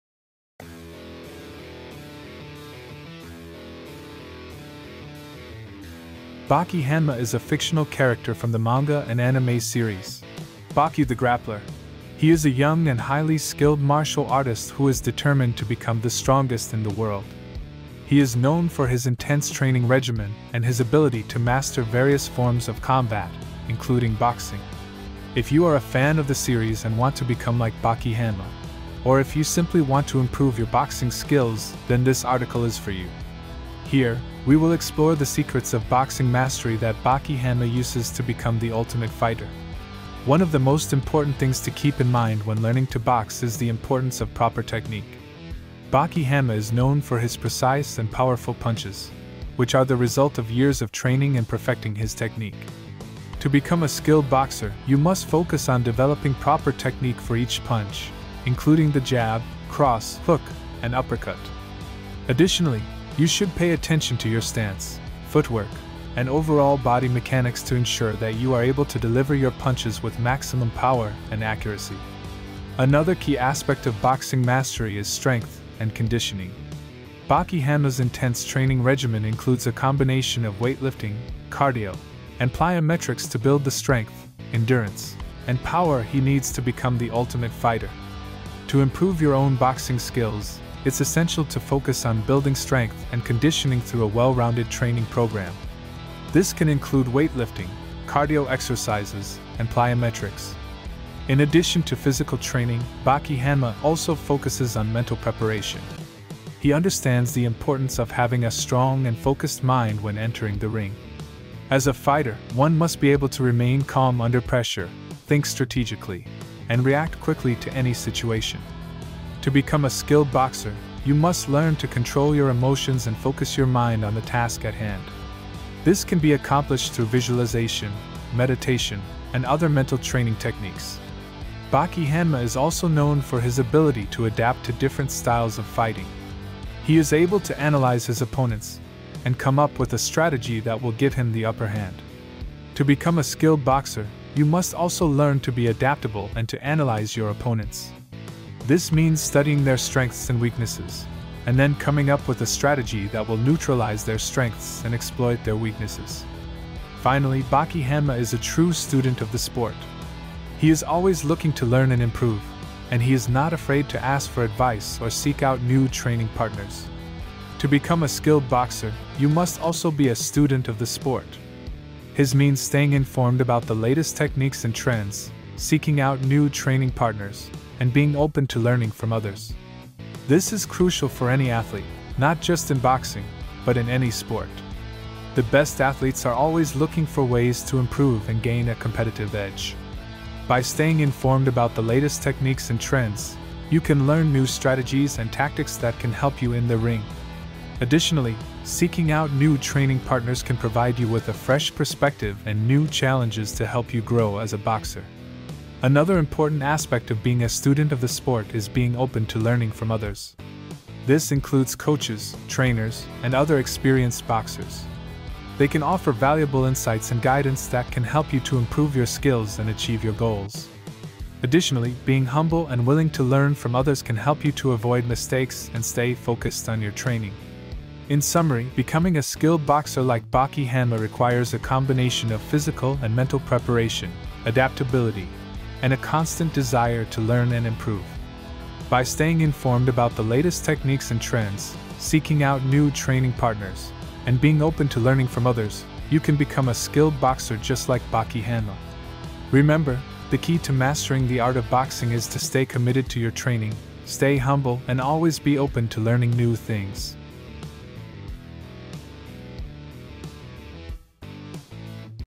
Become the Ultimate Fighter like Baki Hanma: Learn the Secrets of Boxing Mastery
Baki Hanma is a fictional character from the manga and anime series "Baki the Grappler". He is a young and highly skilled martial artist who is determined to become the strongest in the world. He is known for his intense training regimen and his ability to master various forms of combat, including boxing.

If you are a fan of the series and want to become like Baki Hanma, or if you simply want to improve your boxing skills, then this article is for you. Here, we will explore the secrets of boxing mastery that Baki Hanma uses to become the ultimate fighter.

One of the most important things to keep in mind when learning to box is the importance of proper technique. Baki Hanma is known for his precise and powerful punches, which are the result of years of training and perfecting his technique. To become a skilled boxer, you must focus on developing proper technique for each punch, including the jab, cross, hook, and uppercut. Additionally, you should pay attention to your stance, footwork, and overall body mechanics to ensure that you are able to deliver your punches with maximum power and accuracy.

Another key aspect of boxing mastery is strength and conditioning. Baki Hanma's intense training regimen includes a combination of weightlifting, cardio, and plyometrics to build the strength, endurance, and power he needs to become the ultimate fighter. To improve your own boxing skills, it's essential to focus on building strength and conditioning through a well-rounded training program. This can include weightlifting, cardio exercises, and plyometrics.

In addition to physical training, Baki Hanma also focuses on mental preparation. He understands the importance of having a strong and focused mind when entering the ring. As a fighter, one must be able to remain calm under pressure, think strategically, and react quickly to any situation. To become a skilled boxer, you must learn to control your emotions and focus your mind on the task at hand. This can be accomplished through visualization, meditation, and other mental training techniques.

Baki Hanma is also known for his ability to adapt to different styles of fighting. He is able to analyze his opponents and come up with a strategy that will give him the upper hand. To become a skilled boxer, you must also learn to be adaptable and to analyze your opponents. This means studying their strengths and weaknesses, and then coming up with a strategy that will neutralize their strengths and exploit their weaknesses.

Finally, Baki Hanma is a true student of the sport. He is always looking to learn and improve, and he is not afraid to ask for advice or seek out new training partners. To become a skilled boxer, you must also be a student of the sport. This means staying informed about the latest techniques and trends, seeking out new training partners, and being open to learning from others.

In conclusion, becoming the ultimate fighter like Baki Hanma requires a combination of physical and mental preparation, adaptability, and a willingness to learn. By focusing on proper technique, strength and conditioning, mental preparation, adaptability, and a constant desire to improve, you can become a skilled boxer just like Baki Hanma. Remember, it takes time and dedication to master the art of boxing, so be patient and stay committed to your training. With hard work and dedication, you can become the ultimate fighter like Baki Hanma.